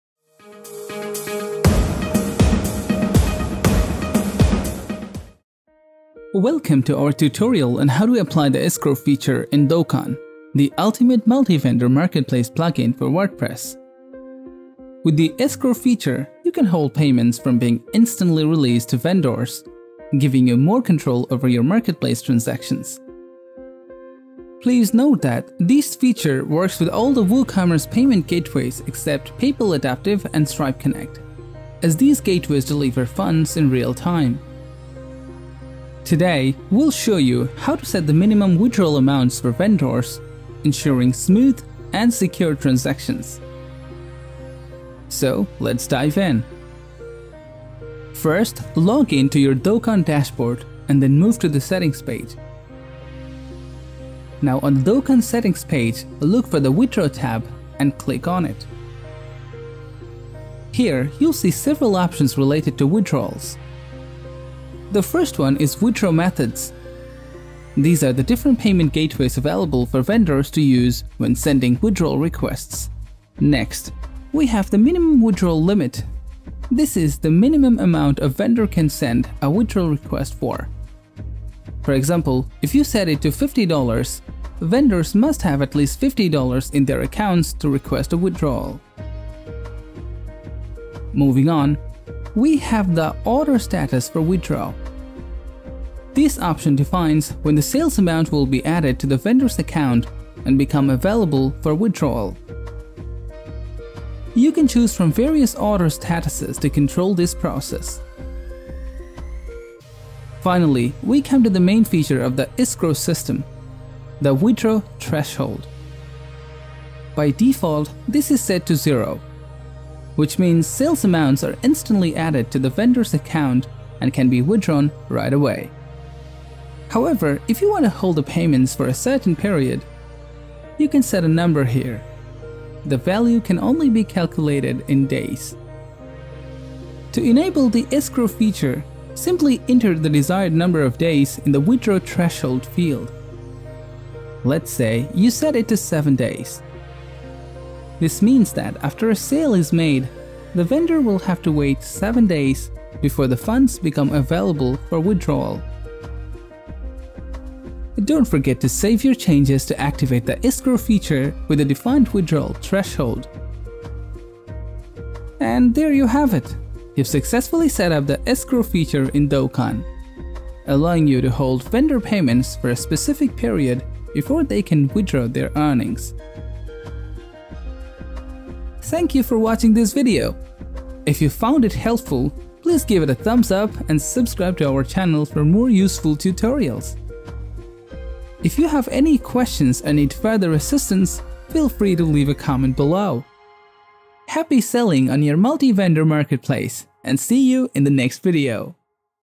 How to Set Up Escrow in Dokan Marketplace
Welcome to our tutorial on how to apply the Escrow feature in Dokan, the ultimate multi-vendor marketplace plugin for WordPress!

With the Escrow feature, you can hold payments from being instantly released to vendors, giving you more control over your marketplace transactions.
Please note that this feature works with all WooCommerce payment gateways except PayPal Adaptive and Stripe Connect, as these gateways deliver funds in real time.

In this video, we'll show you how to set the minimum withdrawal amount for vendors, ensuring smooth and secure transactions. Watch as we guide you step-by-step through the process of enabling the Escrow feature and using the "Withdraw Threshold" to hold vendor payments for a specific period before they can withdraw their earnings.
Key Topics Covered:
1-Understanding the Escrow feature and its importance in building trust.
2-Enabling the Escrow feature in Dokan by adjusting the "Withdraw Threshold."
3-Configuring the "Minimum Withdrawal Limit" and "Order Status for Withdraw" options.
4-Step-by-step guide to setting the desired number of days for holding payments.
4-Saving changes and activating the Escrow feature for enhanced transaction control.

Join us and take full advantage of the Escrow feature to boost confidence among vendors and customers alike, ensuring a secure and reliable multi-vendor marketplace!

To make it more understable here is the video time stamp
✦ ✦ ✦ Time-Stamps ✦ ✦ ✦
▶️  00:00 - Introduction
▶️  00:06 - What is Escrow Feature
▶️  00:47 - Dokan withdraw settings overview
▶️  01:44 - How to setup  Escrow feature
▶️  02:49 - Conclusion

✦ ✦ ✦ Essential Pages of Dokan ✦ ✦ ✦

✦✦✦ Explore more Videos on Dokan ✦✦✦

We're here to support your success in managing your thriving marketplace with Dokan!